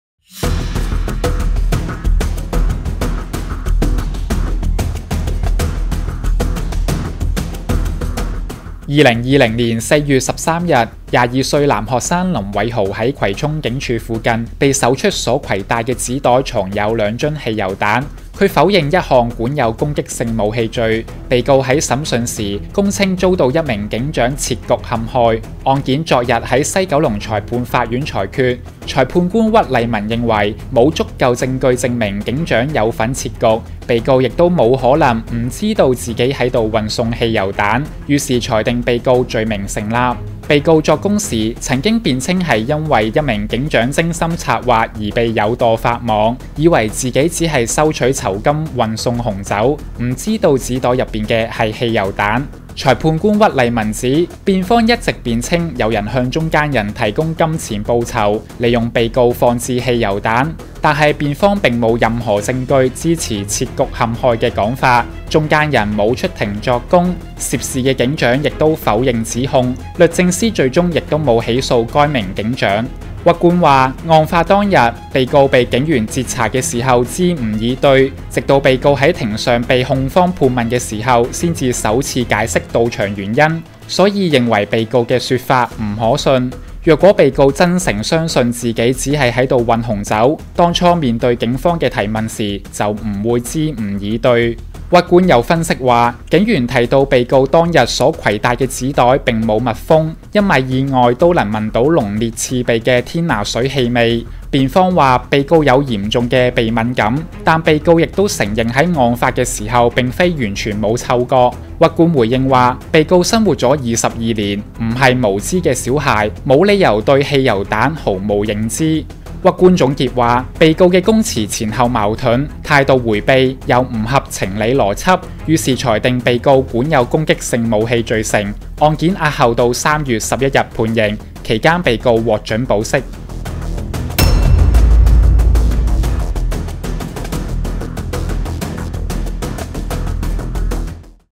【短片】【法官真言】藏汽油彈男學生藏攻擊性武器罪成、辯稱遭警長「設局」、屈麗雯斥：供詞矛盾、22歲非無知小孩!
//汽油彈氣味濃烈，話聞唔到氣味、又話遭警長設局，呢啲講法真係匪夷所思。//